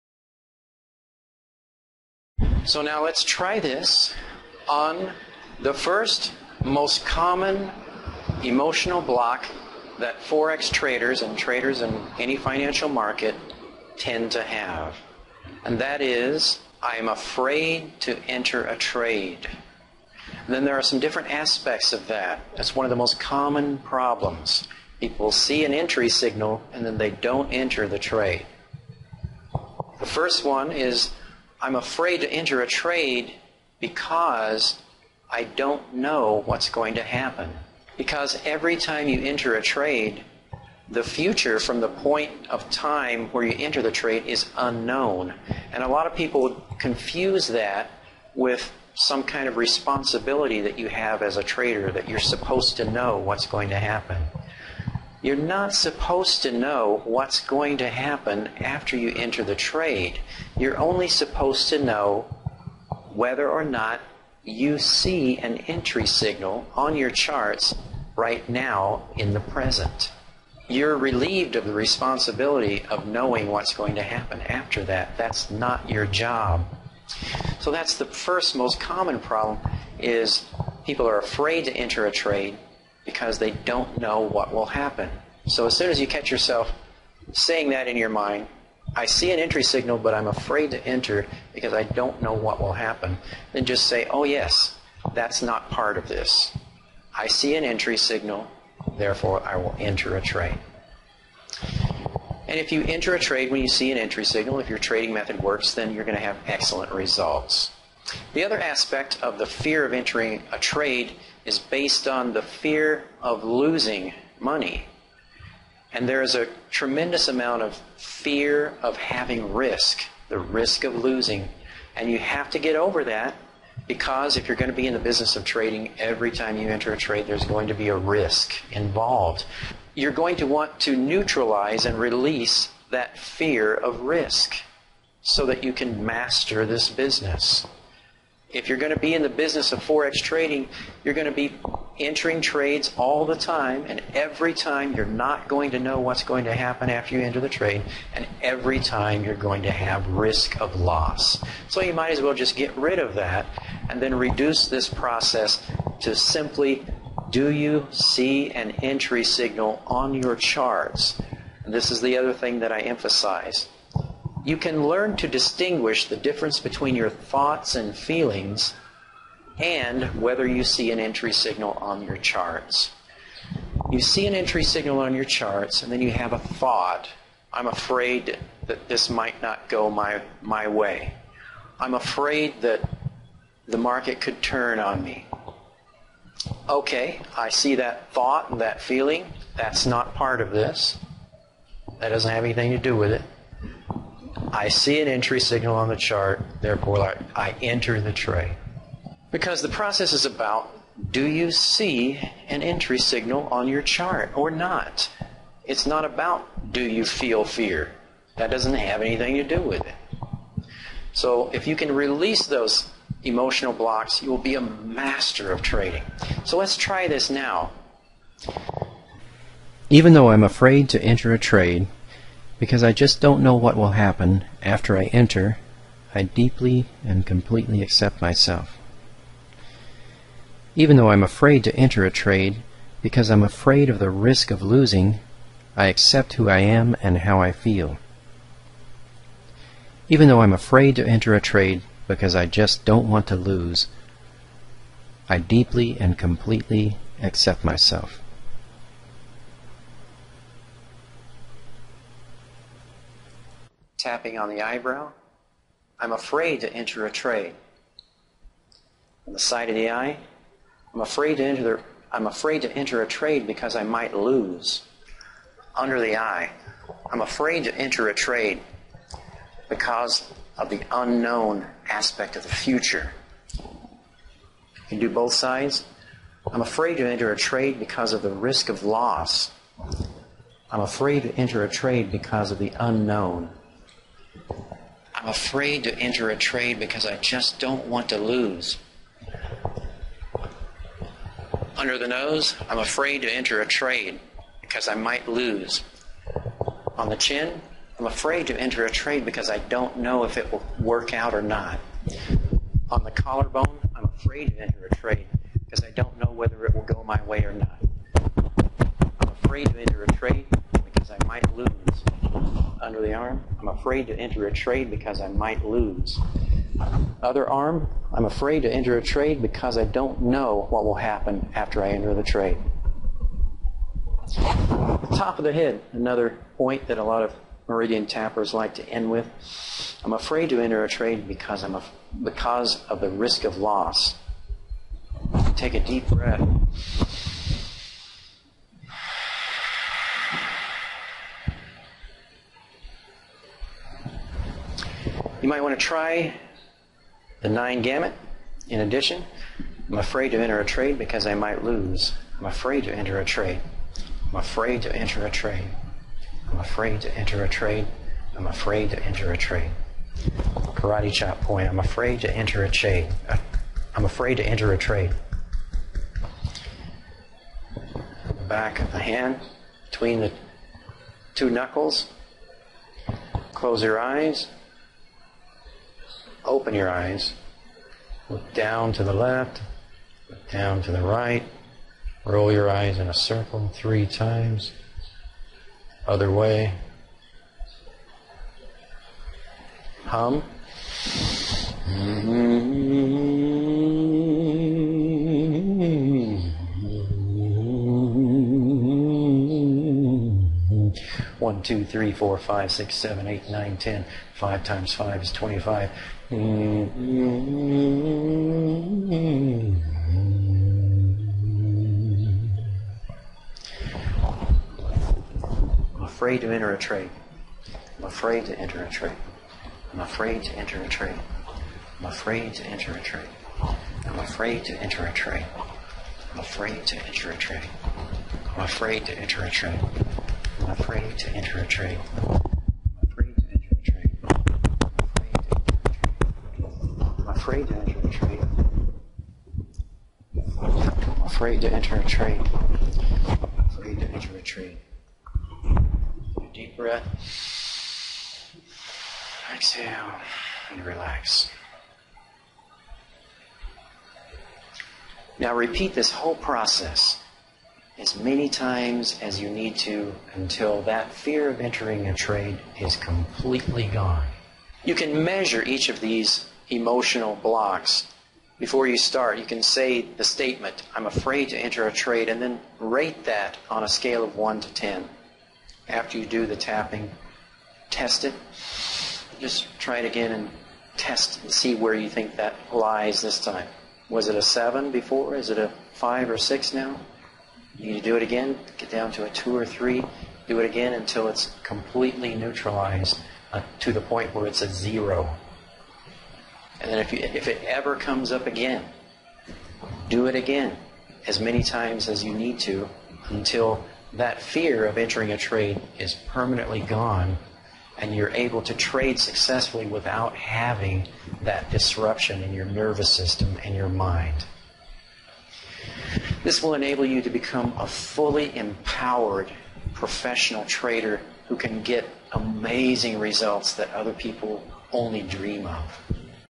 Part 4: Eliminate Emotional Blocks in Trading Using Meridian Tapping
Fourth part of this week's Meridian Tapping video series. Meridian Tapping is an extremely effective method for many people in all walks of life including Forex Traders to remove emotional blocks that prevent them from accomplishing what they intend to do.

As a result, some traders who use Meridian Tapping are finally winning after years of struggling to break through the barrier that prevented them from getting consistent profit..